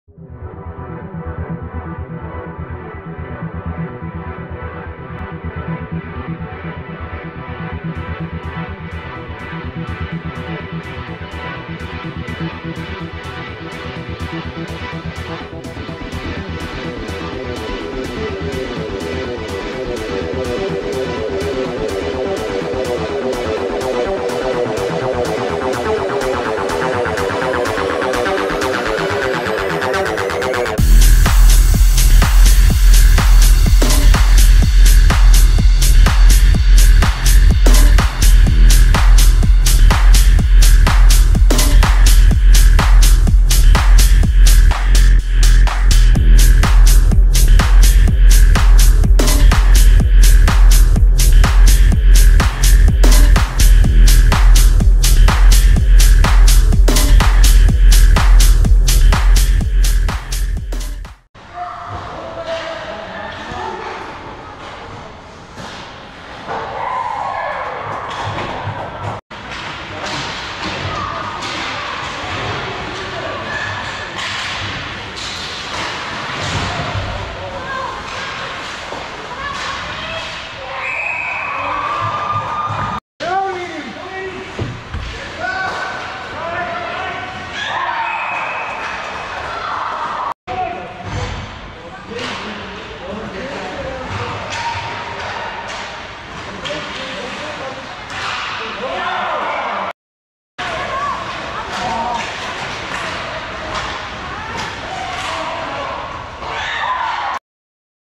2010 HPL Lumberjacks vs Bandits
November 25, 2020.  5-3 win for the Lumberjacks.
11-5-3 record on the year.